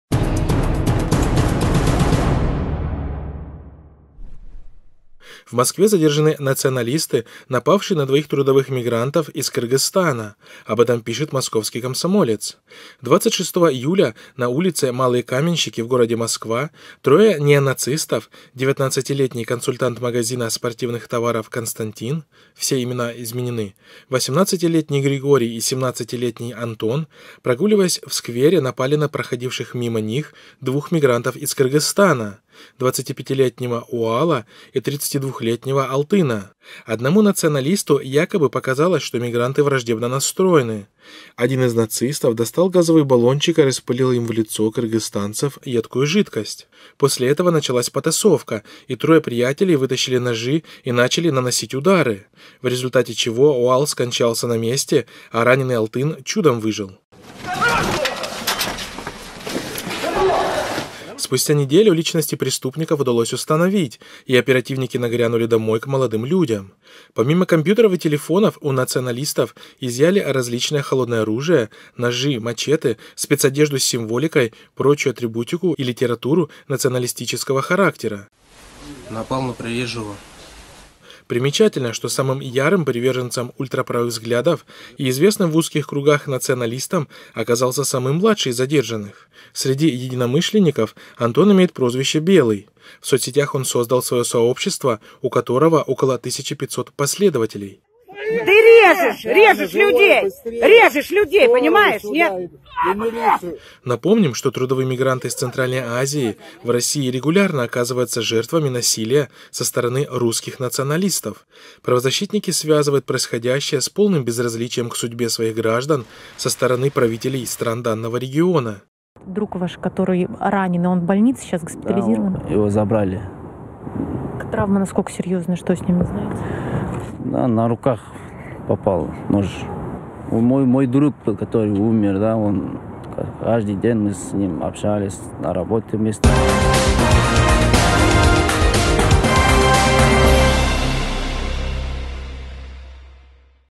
В Москве задержаны националисты, напавшие на мигрантов
В Москве задержаны националисты, напавшие на двоих трудовых мигрантов из Кыргызстана, об этом пишет “Московский Комсомолец”.